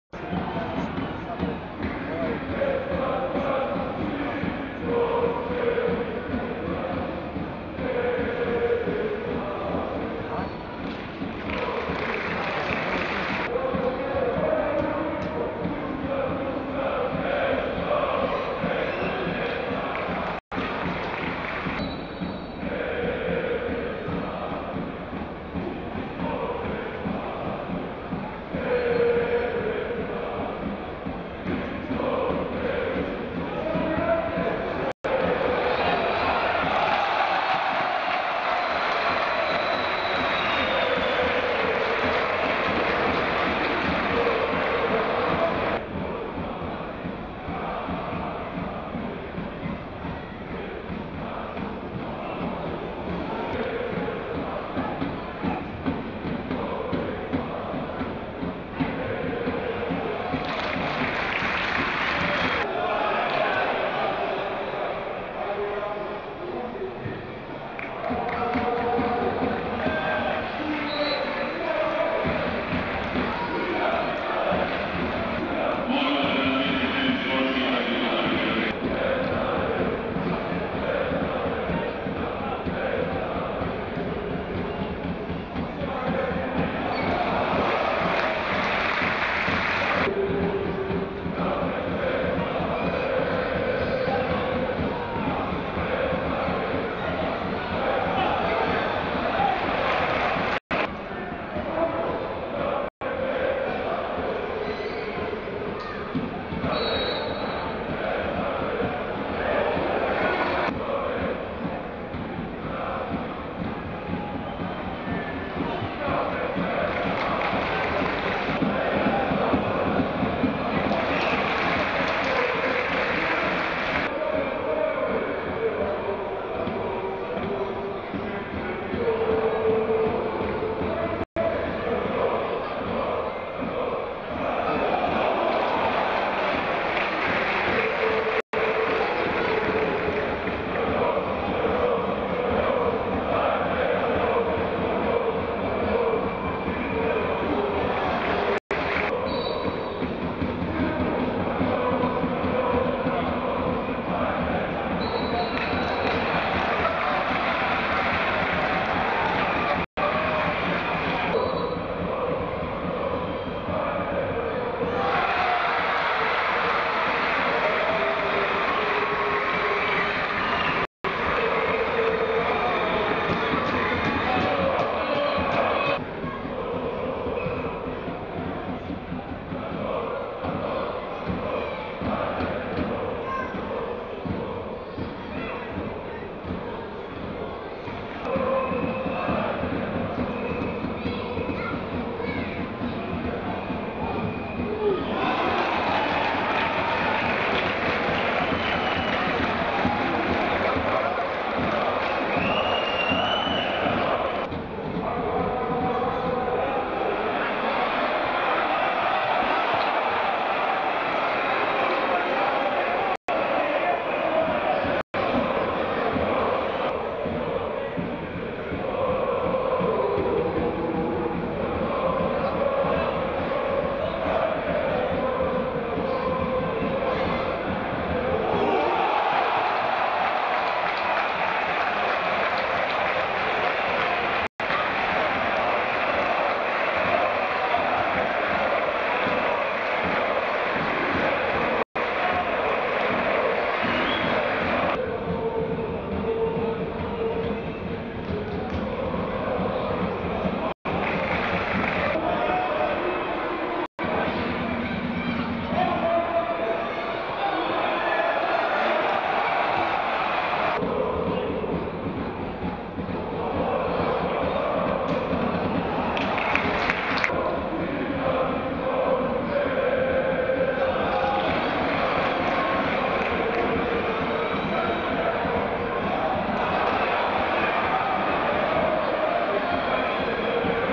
Super liga 2018/19: 10.Kolo: CRVENA ZVEZDA – MLADOST 2:1 (0:0)
Super liga Srbije: 10.Kolo: CRVENA ZVEZDA – MLADOST 2:1 (0:0)
Strelci: Jonathan Barbosa (pen.) 51', Branko Jovičić 68' - Marko Jevremović 65'